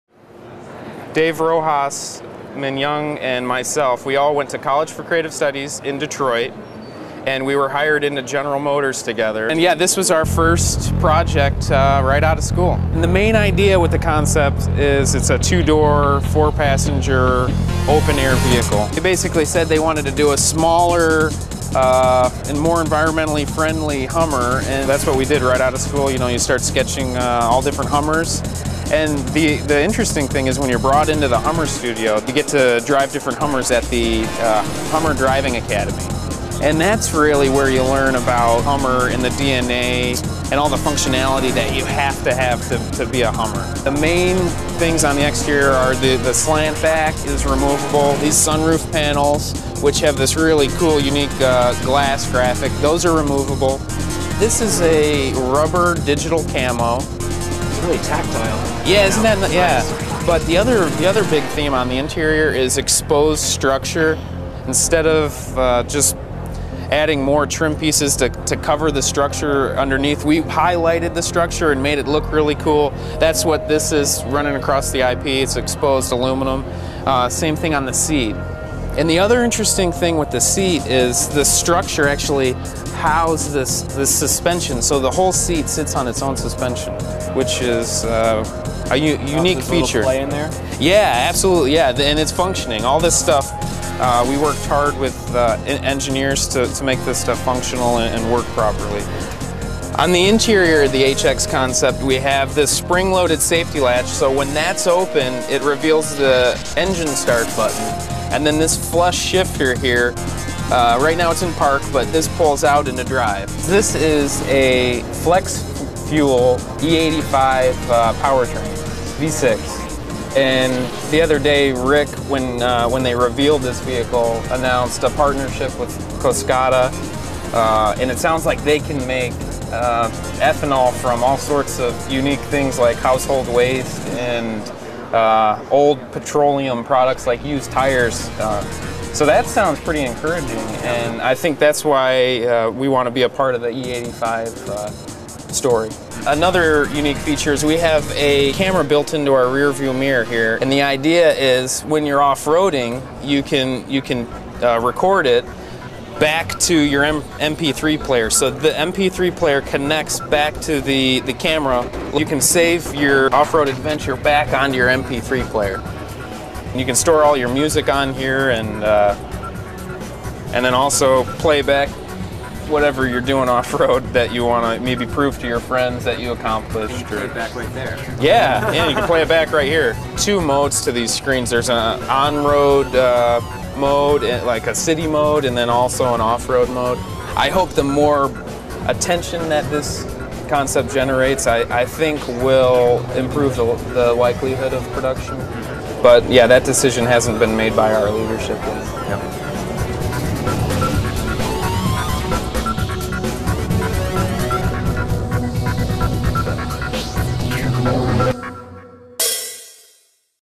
GMnext HUMMER HX CONCEPT
A tour of the Hummer HX concept, provided by design team member Robert Jablonski. This thing looks pretty darn sweet, but the coolest feature is the tactile rubberized digi-print flooring material. Nice! Or perhaps it's the flip-out holder for your iPod, which can capture trail video thru a mirror mounted camera.